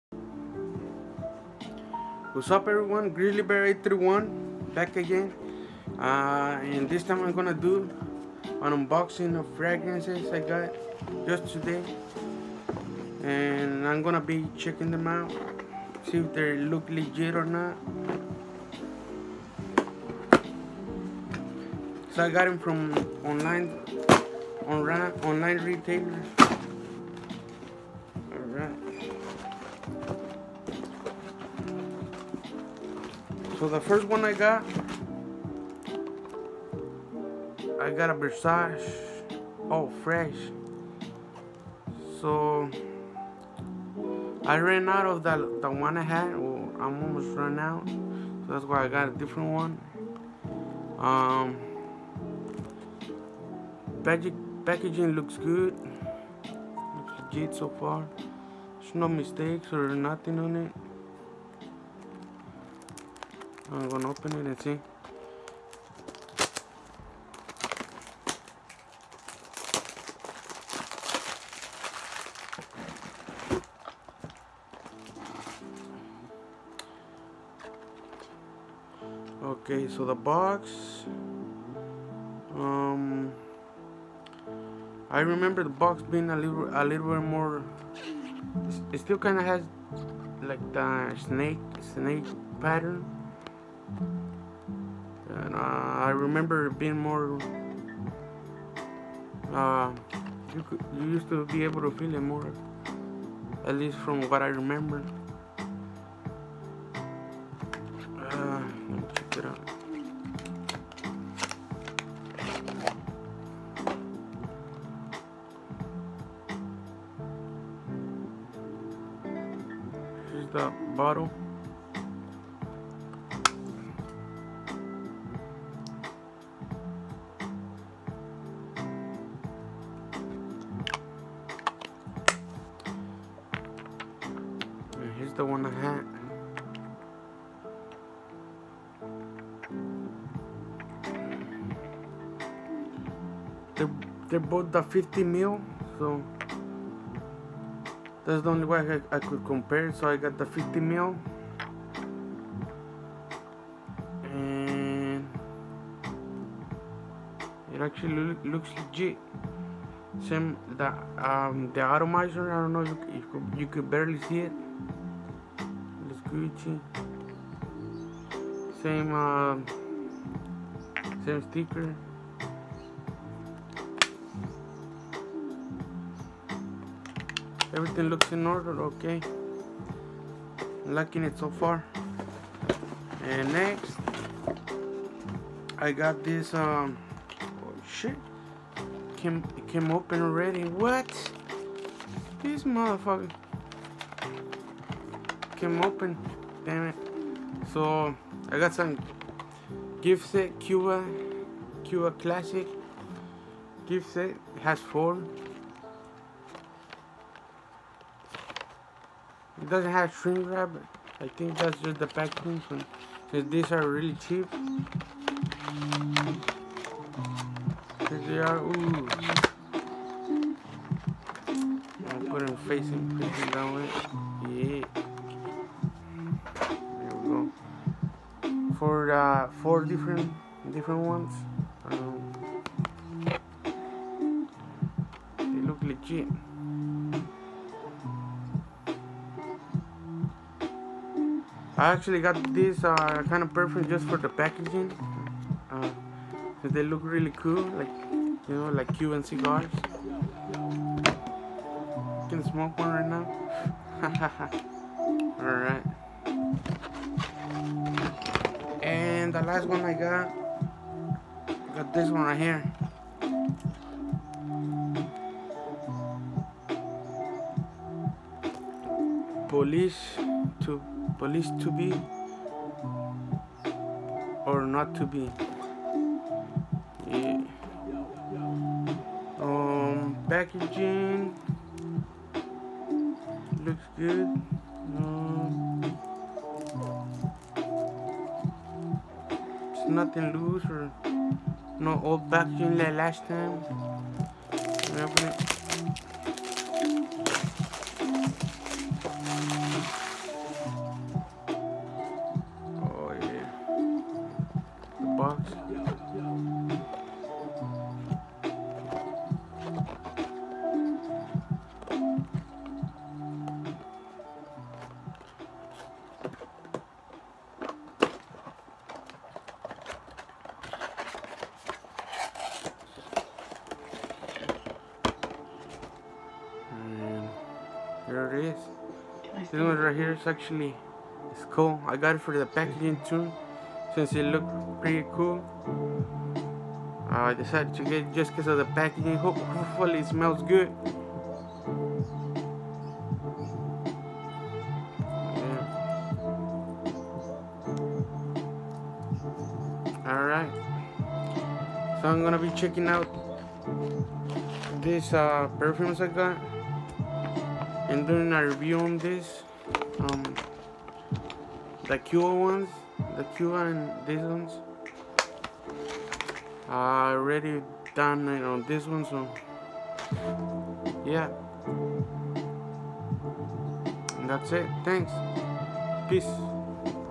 Fragrance unboxing and first impressions 2
Unboxing of some fragrances from FragranceX and first impressions